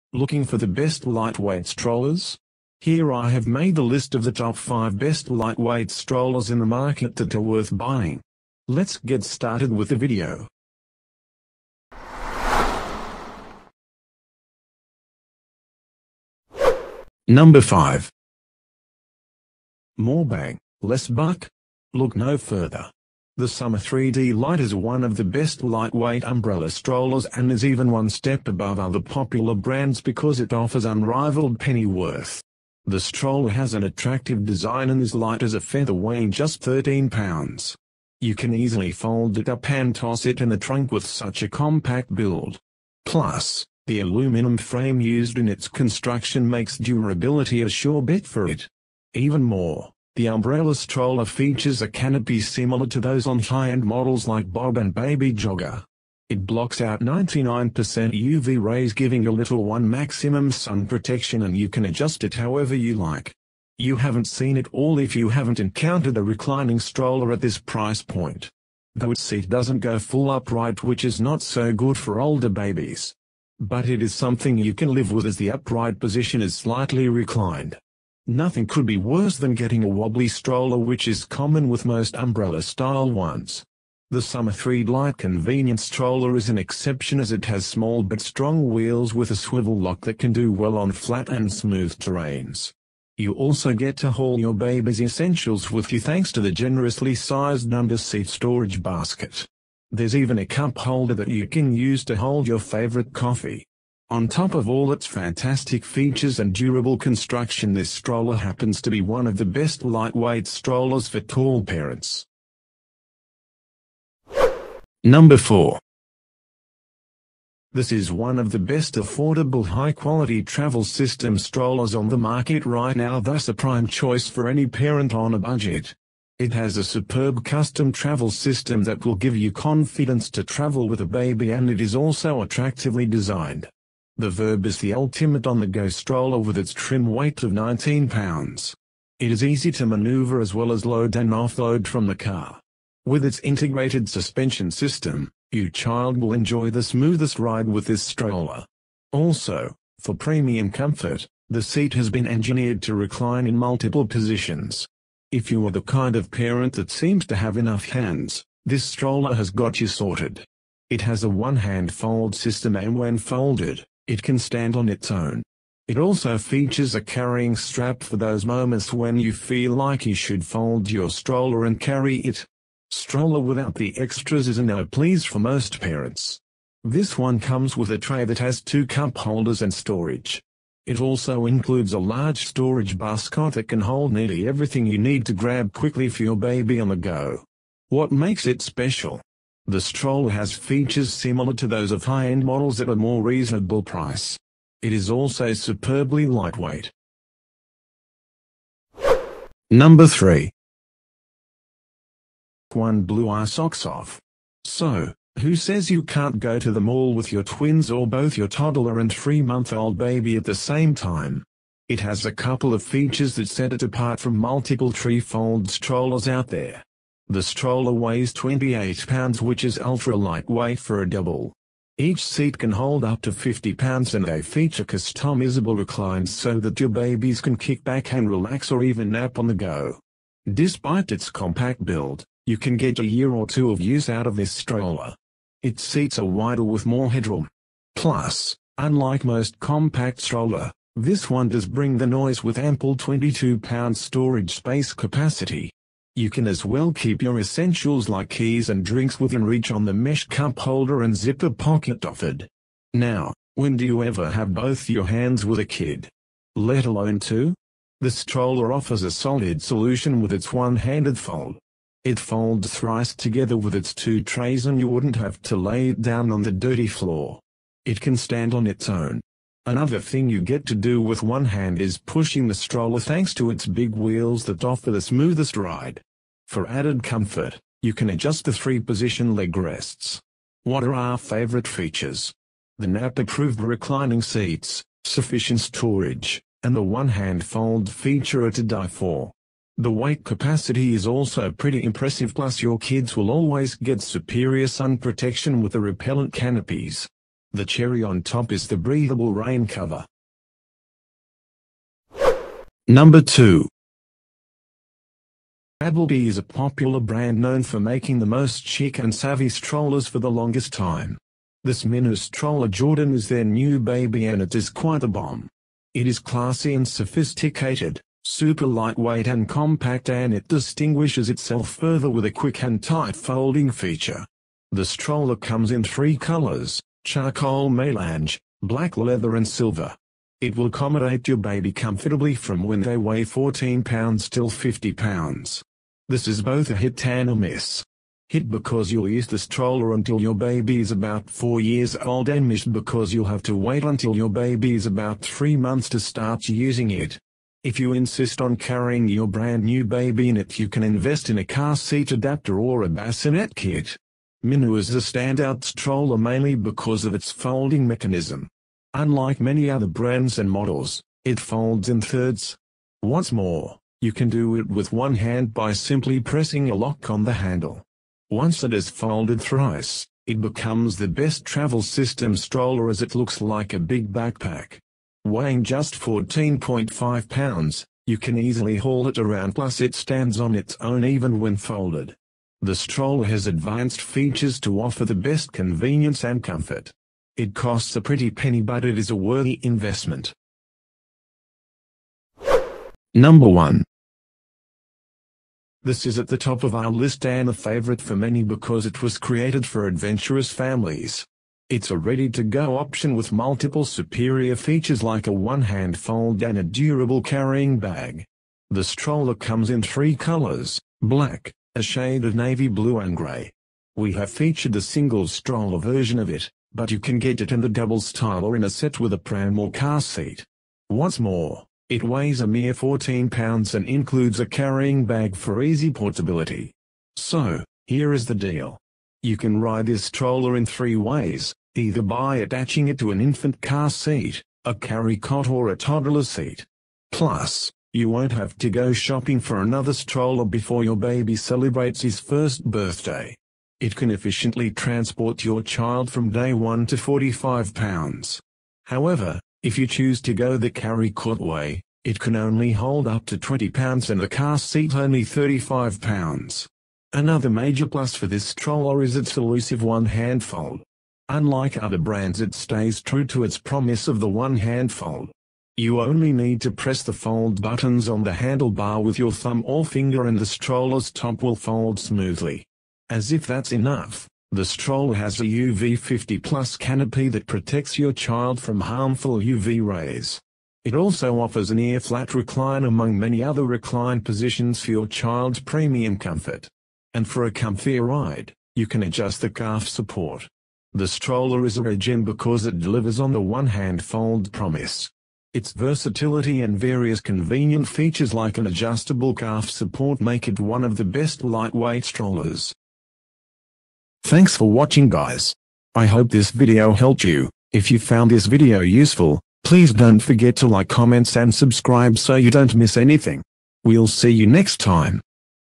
Top 5 Best Lightweight Strollers Review of 2024
In This Video, We Recommended the Top 5 Best Lightweight Strollers 2024

👉05.High Landscape Baby Stroller 3 in 1 With Car Seat